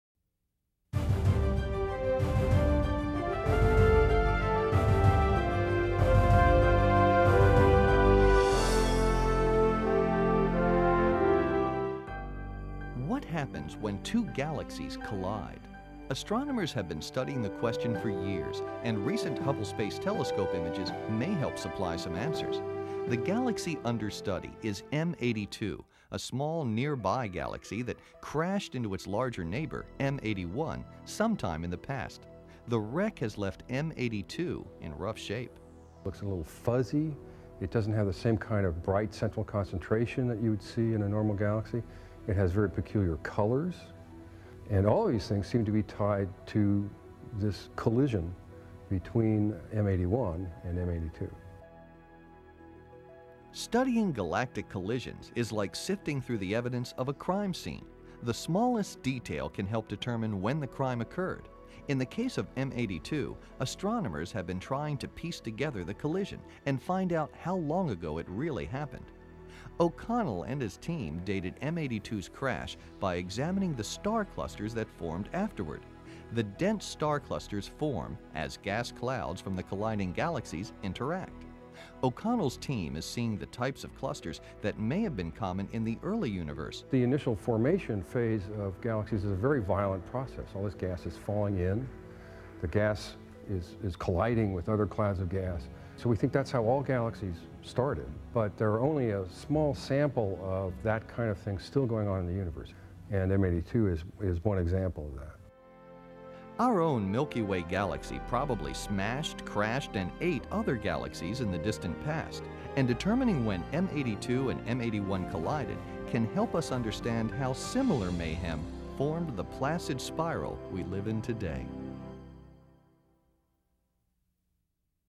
2003: Hubble sees galaxies collide
Discover what happens when galaxies crash into one another, and how astronomers dissect the facts from the clues left behind.